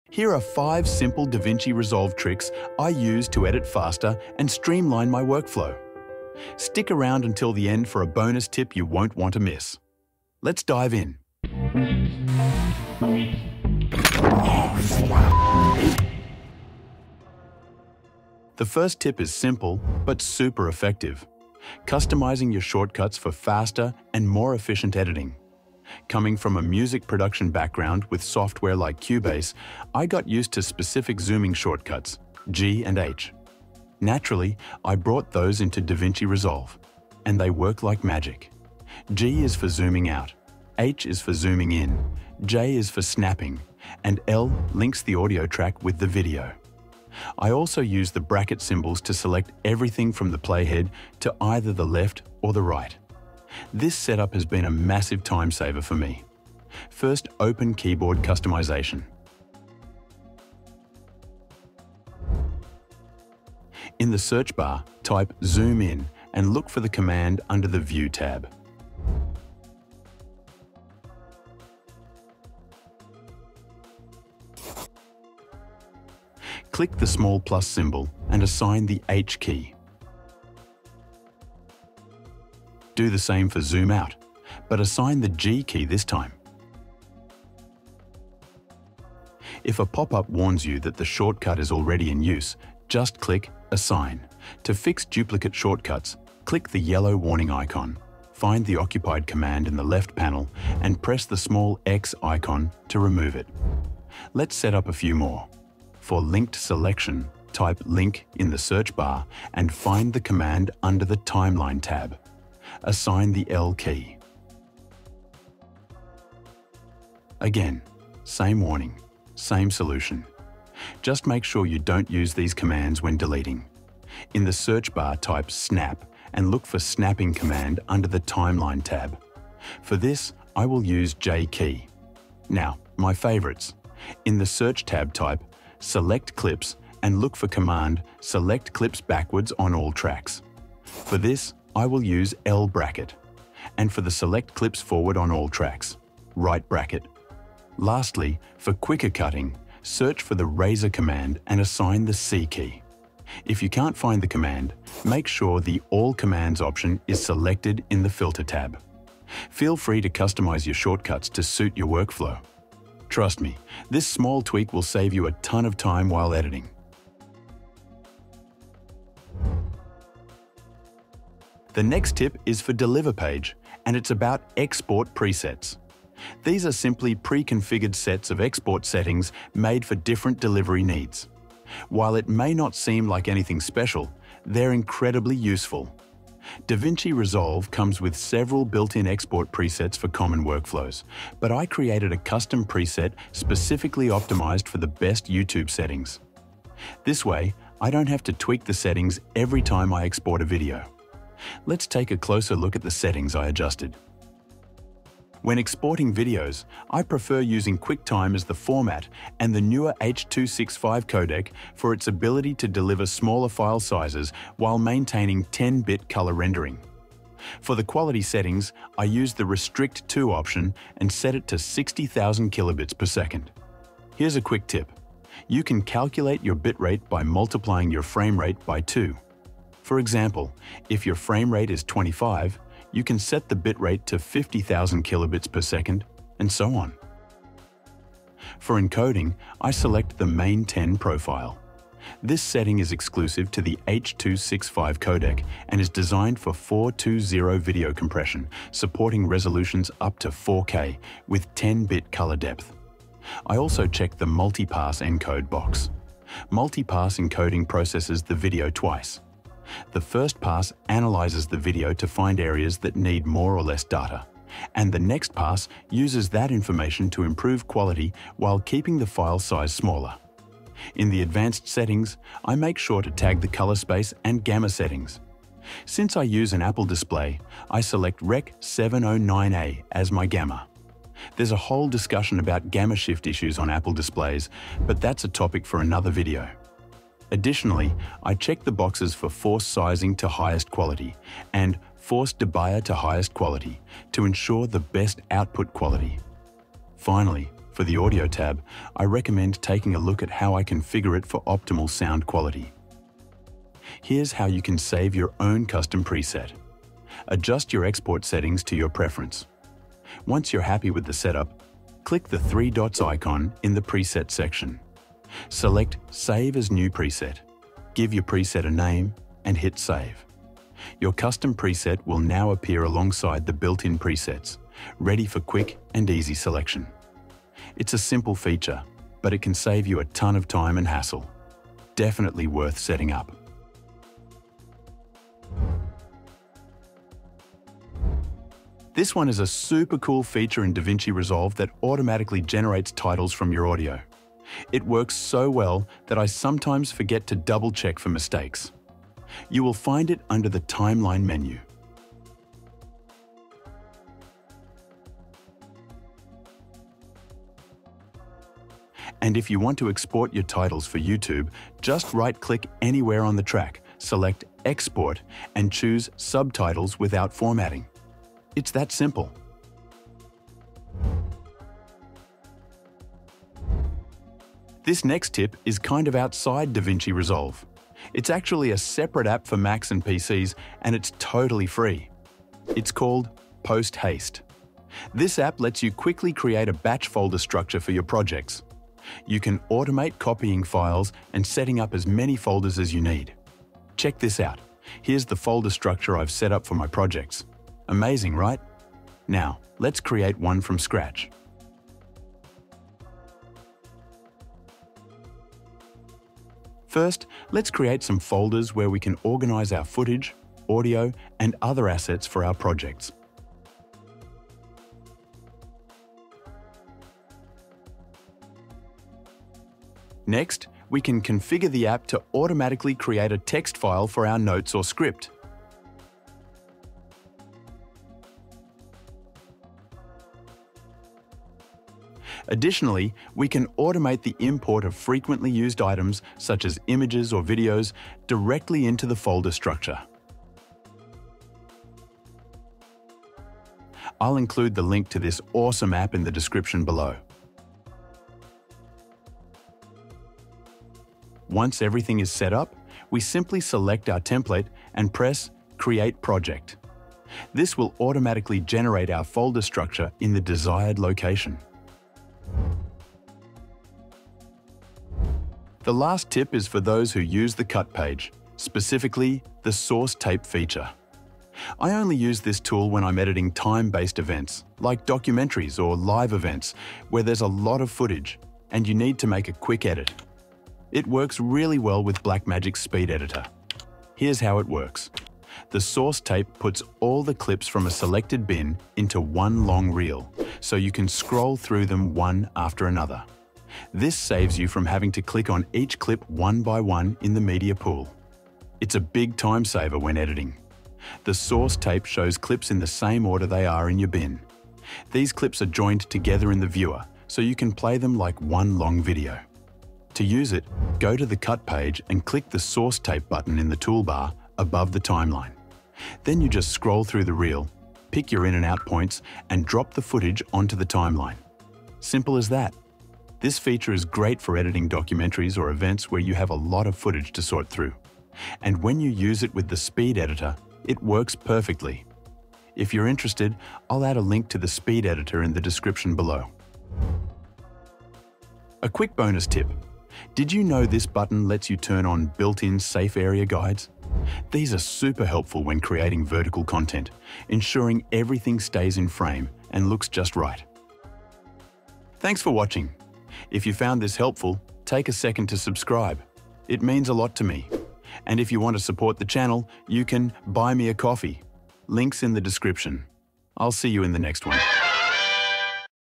5 Editing Tricks That Seem Simple But Will Blow Your Mind!
In this video, discover 5 simple DaVinci Resolve tips to edit faster, plus a bonus trick to streamline your workflow—perfect for creators of all levels!

Davinci Resolve Speed Editor:
🇪🇺Amazon EU ➡️
🇺🇸Amazon US ➡️

Timestamps ⤵️
00:00 Intro
00:19 Tip 1
02:48 Tip 2
05:34 Tip 3
06:10 Tip 4
07:41 Tip 5
08:56 Bonus tip
09:12 Outro

Media used ⤵️
Music (Intro): Liborio Conti